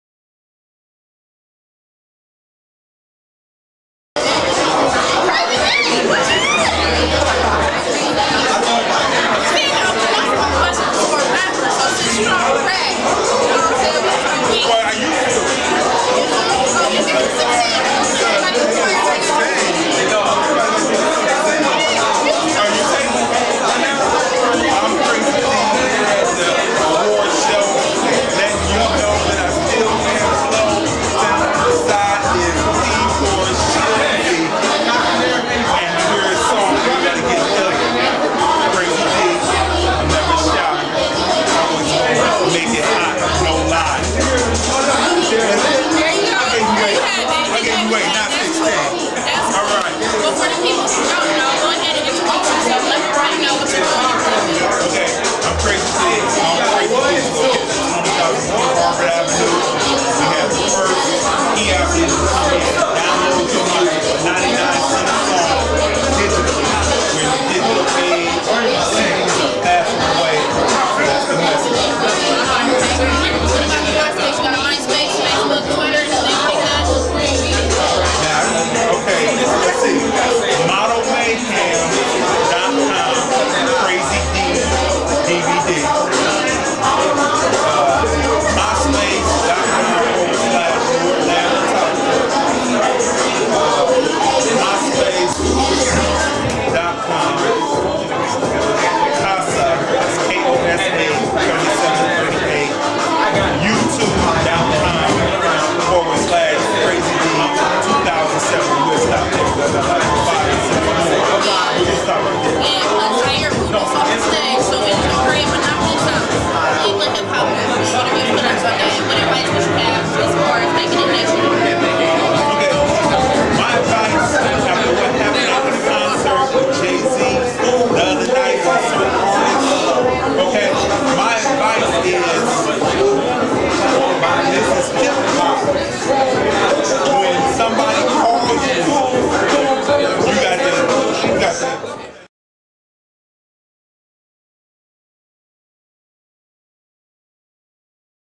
CRAZY DEE AT THE REBEL ARMY RADIO AWARDS
Crazy Dee attended The Rebel Army Radio Awards at The Symposium on West 117 and Detroit Ave., on October 24th, 2009.  He brought with him his Crazy Dee's Muzic Palace located at 20003 Harvard Ave. humor and personality!  This is an interview for Snack TV and can be seen in its complete version on Snack TV.  Crazy Dee is asked to kick a 16, is asked about his store, and his many websites.  He is also asked about the advice he would give to artists trying to take it nationwide, and his answer involves a situation which took place at the Jay-Z concert in Cleveland just a few days before.  A must watch clip, Enjoy!!